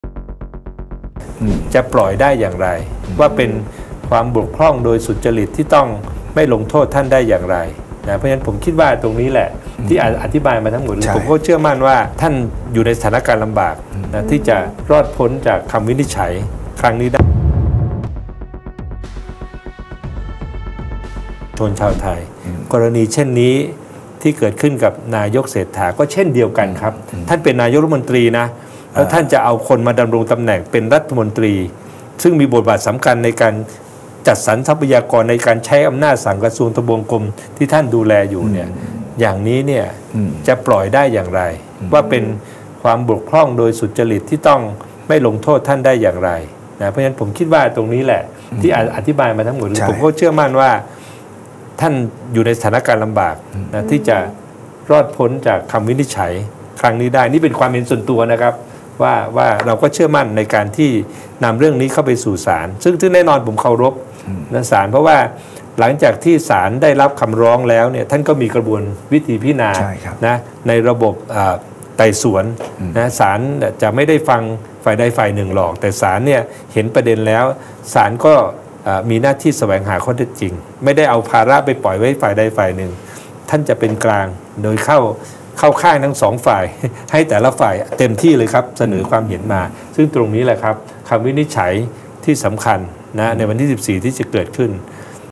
เศรษฐา ควรหลุดนายกฯ
ปมแต่งตั้งคนแบบรัฐมนตรีพิชิต
จะเรียก บกพร่องโดยสุจริต ได้อย่างไร